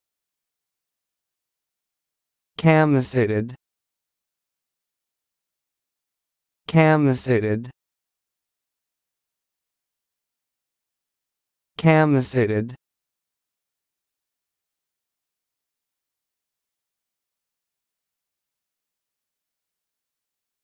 Learn how to say this word: "Camisated"
This is the definition of "Camisated":
a.  Dressed with a shirt over the other garments.